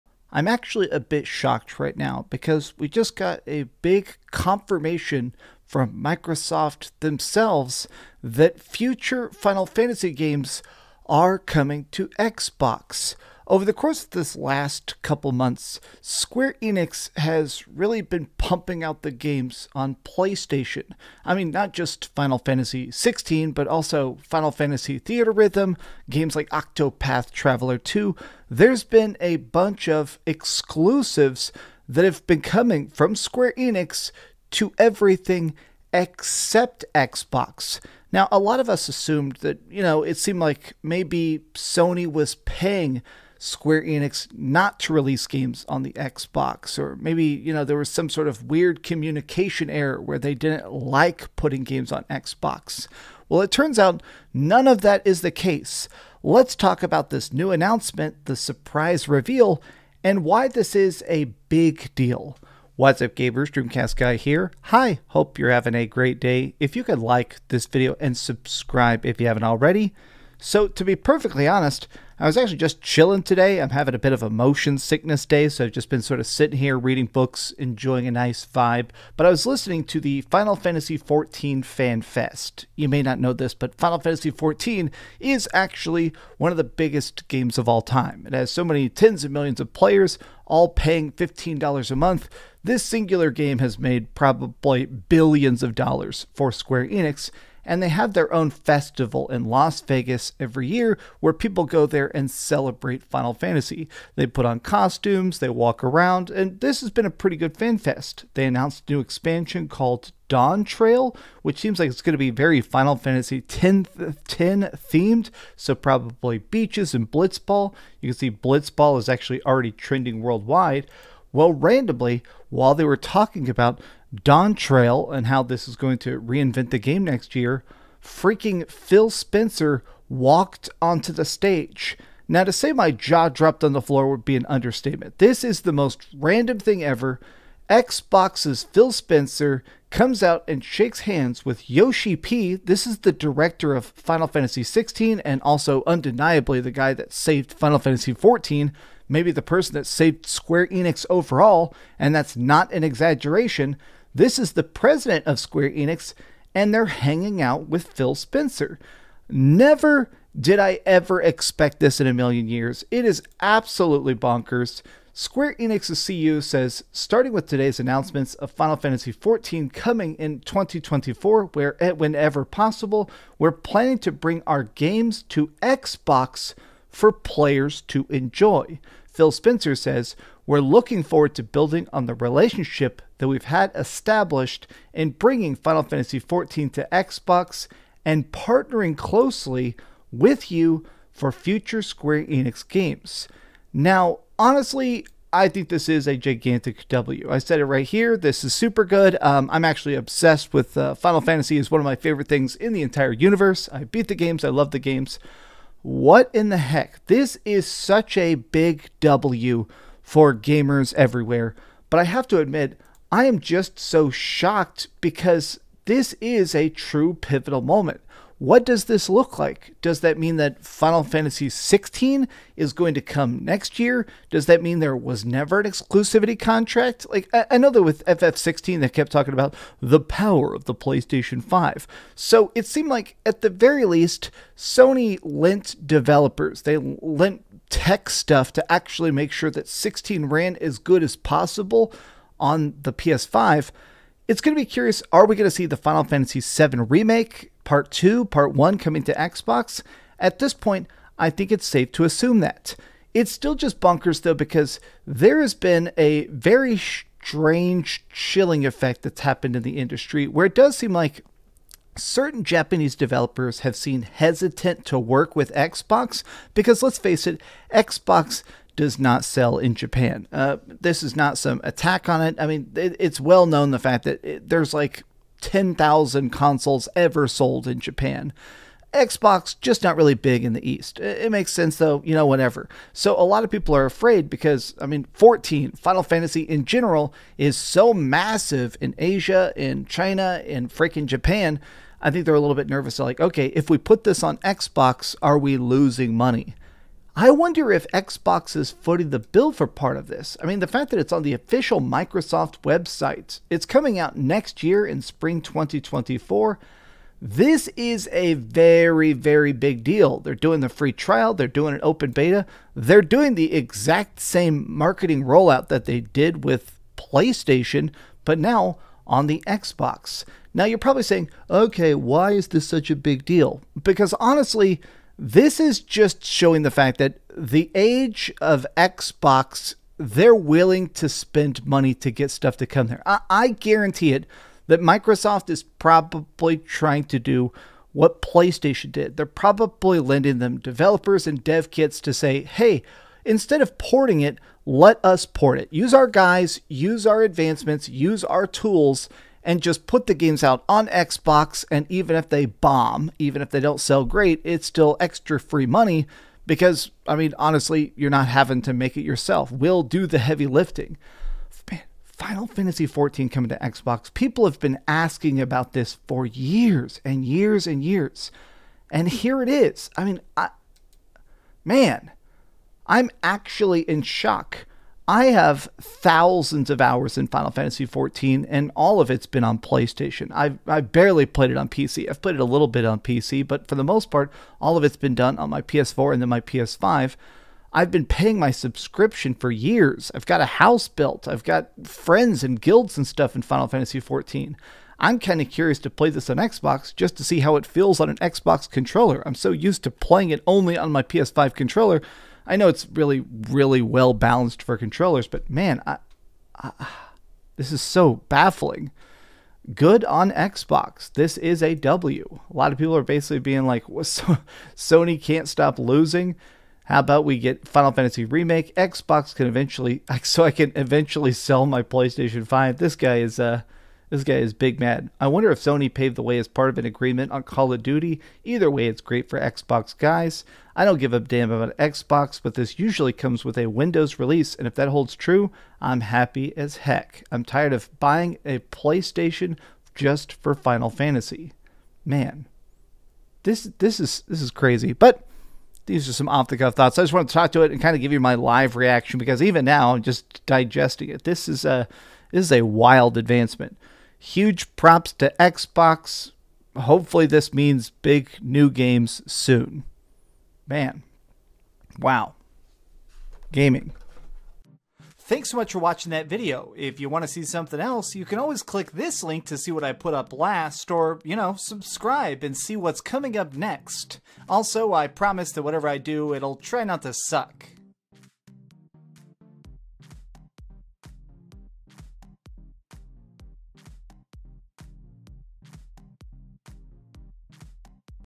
Final Fantasy is coming to XBOX?! Square Enix makes a DEAL with MICROSOFT!
Spencer continued, “The entire Xbox team is excited to see how our newest Warriors of Light will use the full capabilities of Xbox Series X and Series S to help save Eorzea. And I’m excited to partner with you, Yoshida-san, and your studios as you bring this incredible franchise to global platforms to enable more players to connect and experience the joy of gaming together.”

Kiryu then joined Spencer and Yoshida on stage.

“First off, I have to thank you, Phil-san, for working with us to make the Xbox version of Final Fantasy XIV a reality,” Kiryu said. “This is a project that wouldn’t have been possible without [Phil’s] support.”

Kiryu continued, “I think I can speak for myself, [Yoshida], and all of our Warriors of Light here watching the live stream when I say how excited I am to see Xbox support added, and for us to welcome countless new friends to the world of Final Fantasy XIV. Final Fantasy XIV is only going to get bigger and better, so all the Xbox gamers out there, I urge you to give Final Fantasy XIV a try.

“And one more thing, as CEO of Square Enix, we want to continue to deliver fabulous games to fans across the world, and of course we want to welcome the Xbox community as well, and starting with today’s announcement of Final Fantasy XIV coming in spring 2024, and whenever possible, we are planning to bring our games to Xbox for players around the world to enjoy. We’re really looking forward to working closely with Phil and the Xbox team to make this possible.”

Spencer then added, “Thank you, Kiryu-san. We deeply respect the rich legacy of Square Enix, and we look forward to building on the relationship that we’ve established in bringing Final Fantasy XIV to Xbox, and partnering closely with you and the Square Enix team on future games.”

Yoshida closed, “I am extremely thankful and we are committed to working with Xbox going forward.”